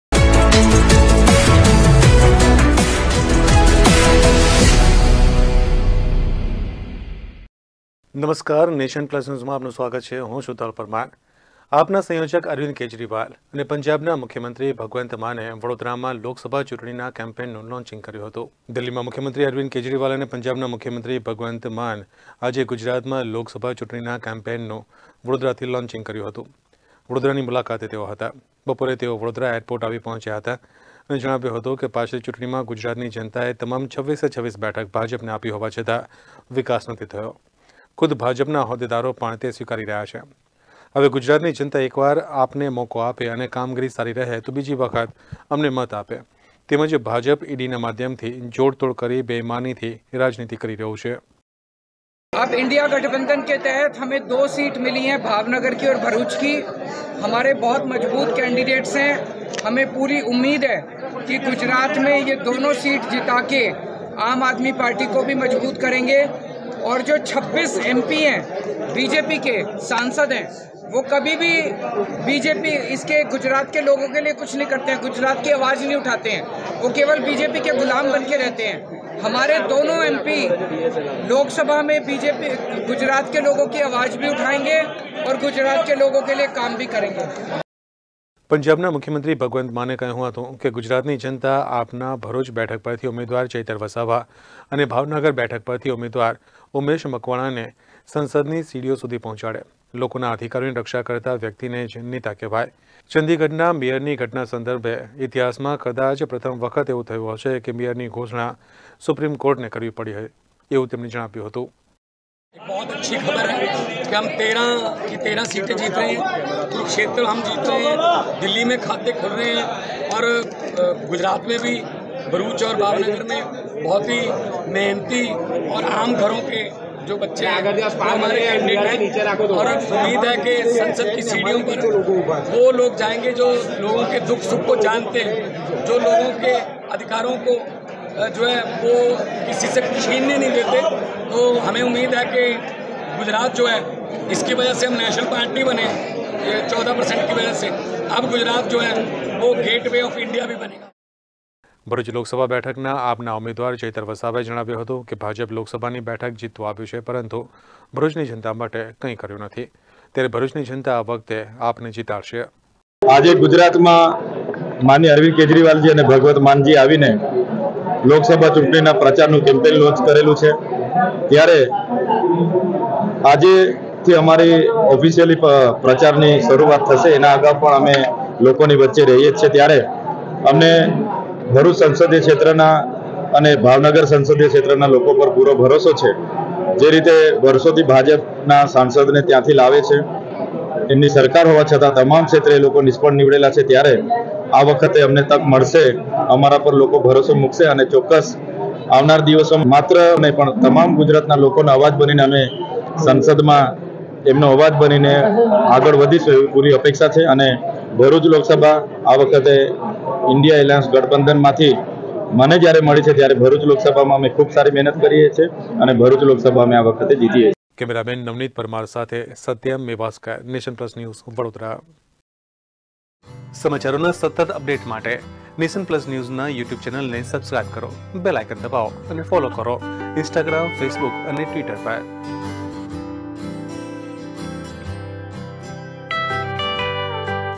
લોકસભા ચૂંટણીના કેમ્પેઈનનું લોન્ચિંગ
આપના સંયોજક અરવિંદ કેજરીવાલ અને પંજાબના મુખ્યમંત્રી ભગવંત માને વડોદરામાં લોકસભા ચૂંટણીના કેમ્પેઈનનું લોન્ચિંગ કર્યું હતું.